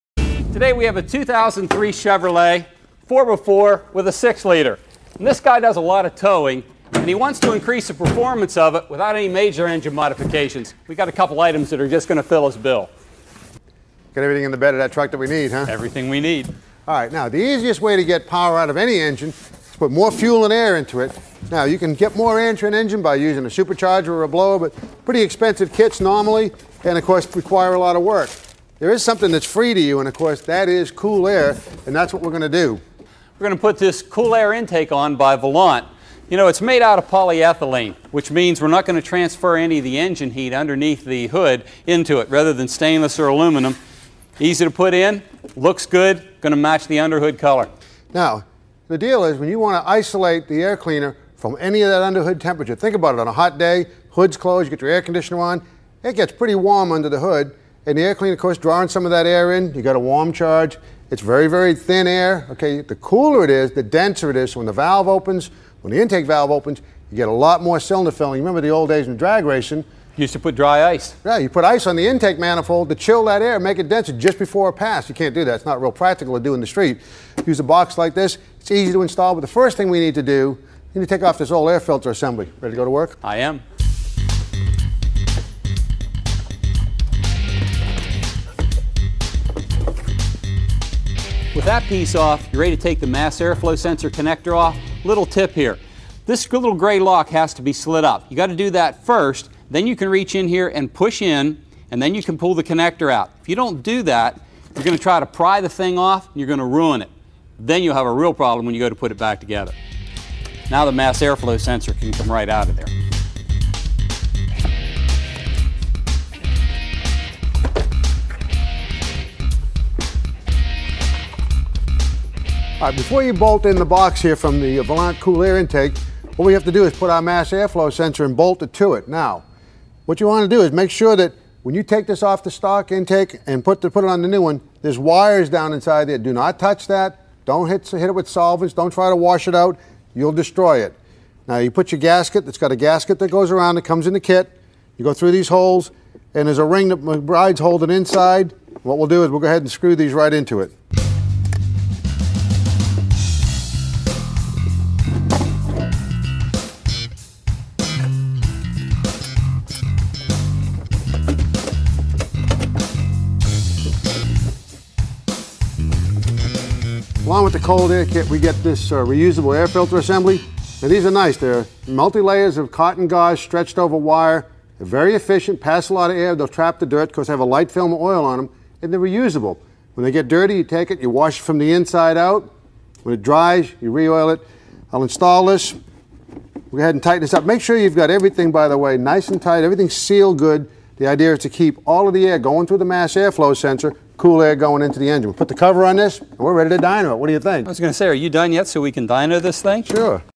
2003 Chevy Silverado Volant Air Intake Installation
Detailed instructions on how to install a Volant Cold Air Intake onto a 2003 Chevy Silverado 4X4 with the 6.0 liter V8 Vortec engine.